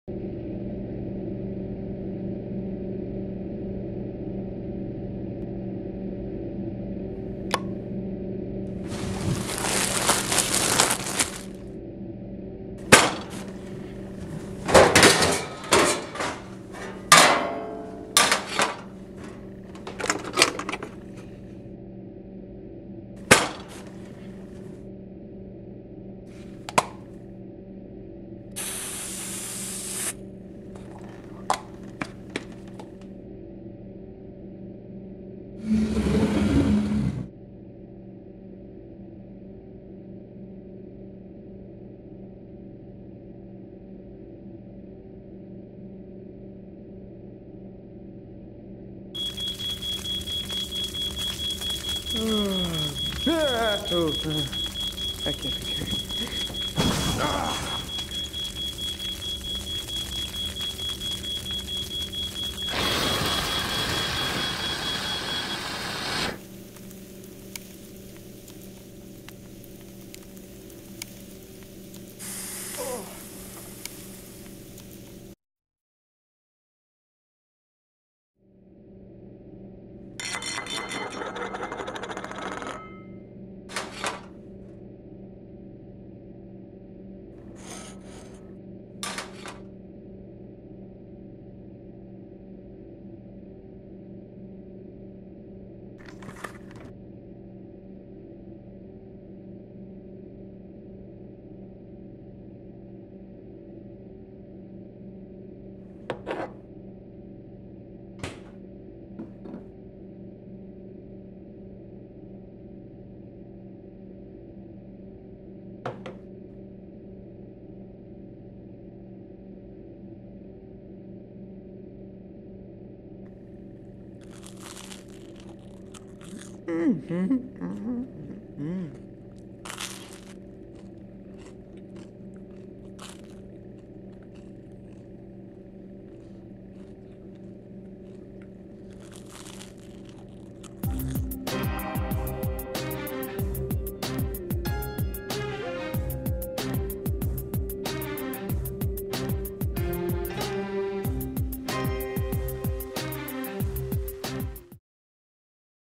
ZEBRAFILM STUDIOS - Midnight Snack
A midnight snack goes awry.